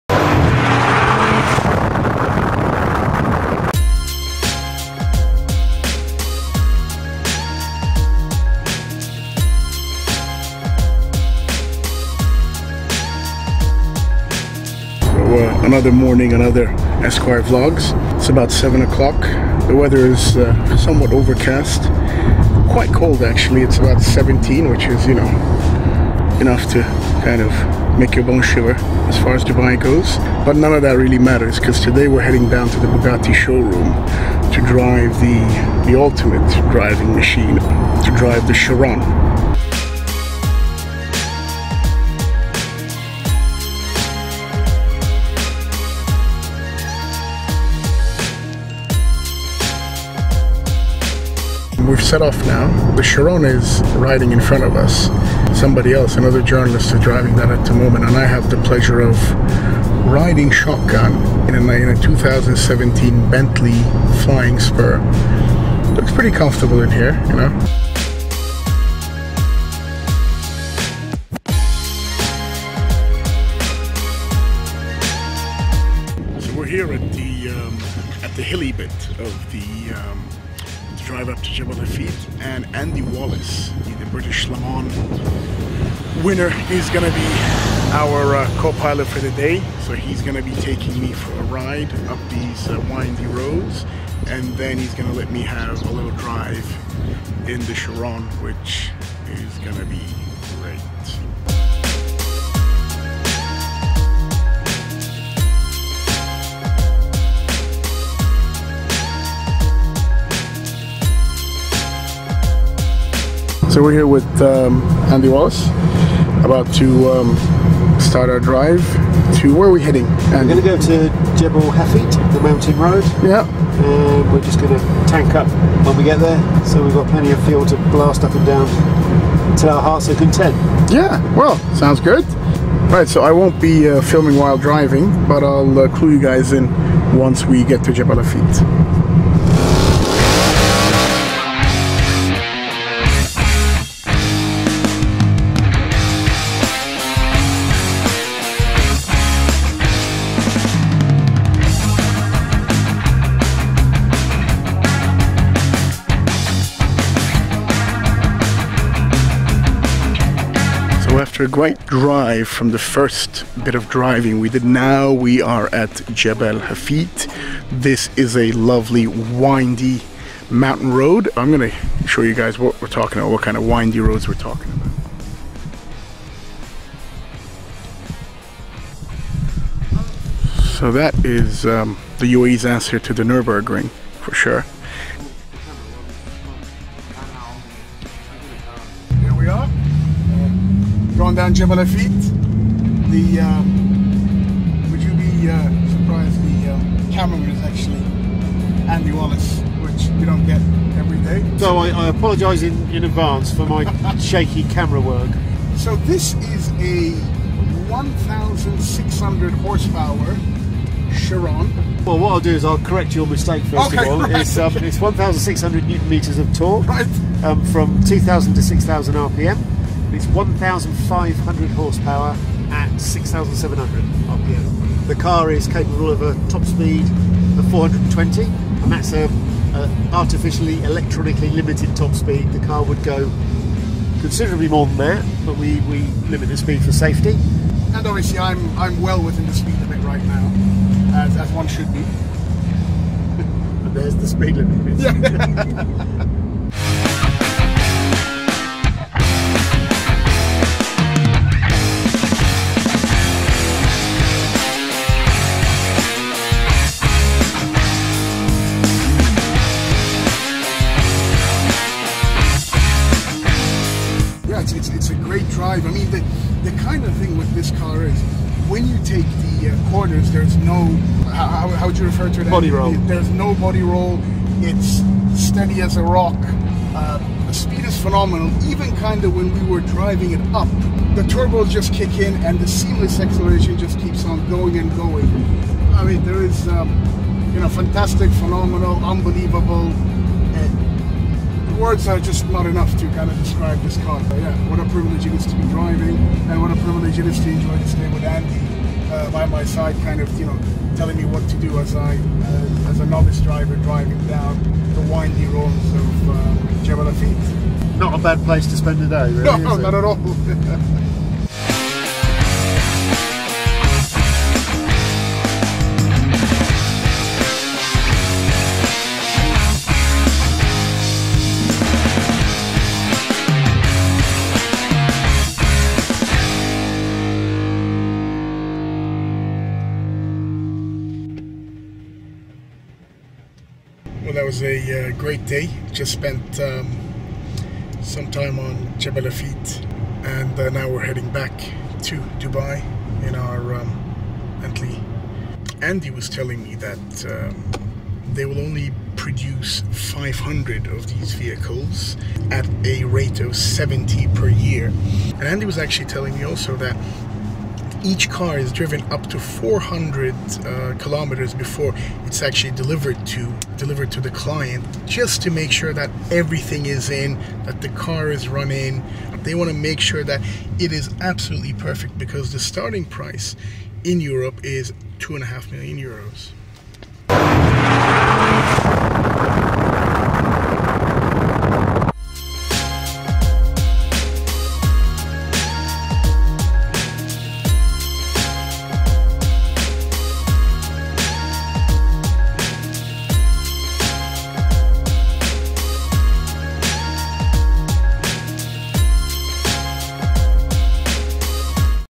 Test driving 420kph Bugatti Chiron in Abu Dhabi Esquire Vlogs 1
Esquire Middle East sends Intrepid Vlogger Alp Sarper to drive the 420kph Bugatti Chiron - 24 Hour Le Mans legend Andy Wallace is the co-pilot.